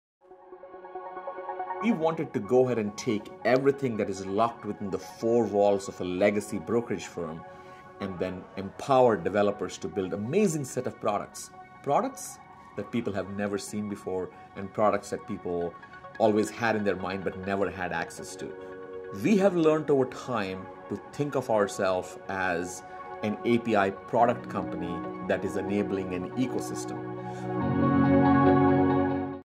Tradier: Enabling an API Ecosystem
Tradier's Dan Raju explains how his firm uses APIs to empower developers to build "amazing products that people have never seen before."